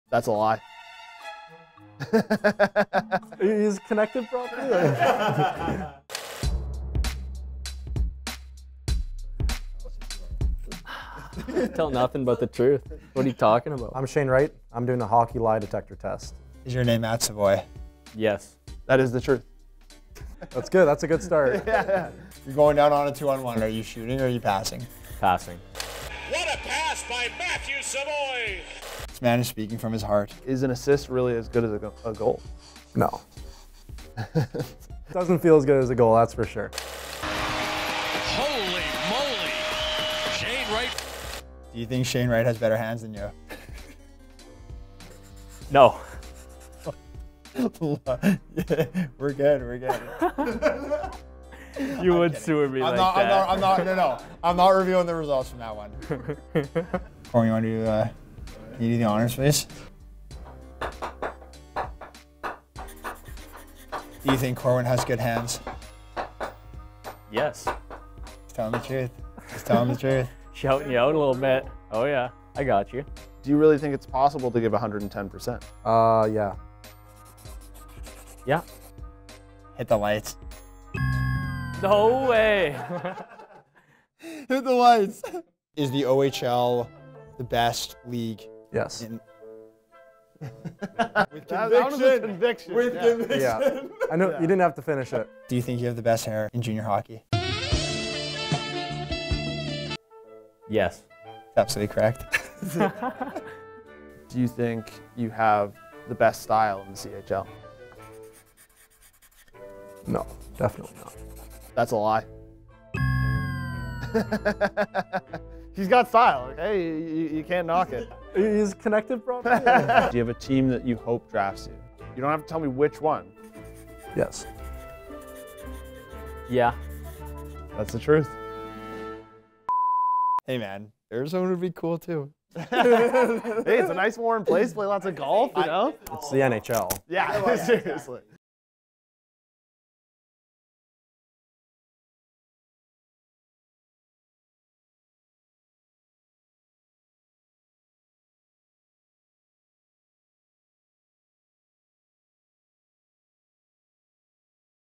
TOP NHL PROSPECTS TAKE LIE DETECTOR TEST
Corwin and Jesse sit down with top NHL prospects Shane Wright & Matt Savoie to put them through a lie detector test. What will they reveal under interrogation?